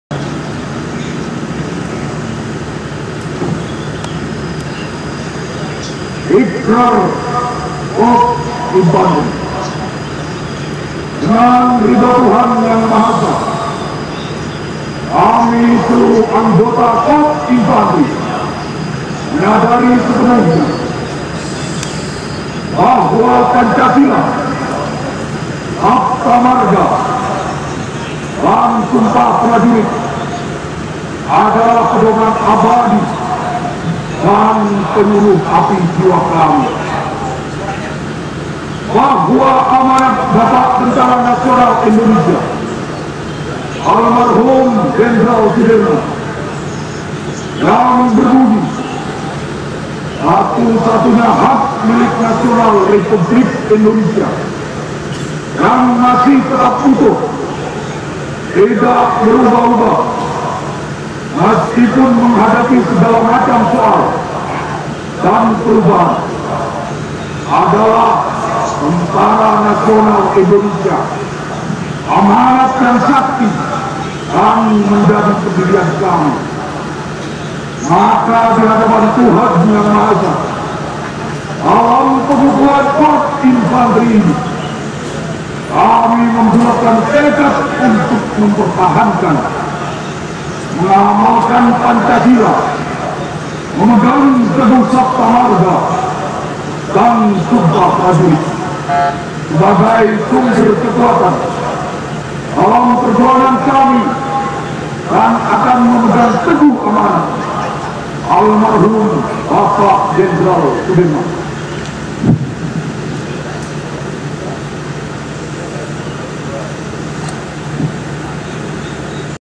IKRAR KORPS INFANTERI (HUT INFANTERI 2022)
Ikrar Korps Infanteri Yudha Wastu Pramuka pada upacara HUT Infanteri tahun 2022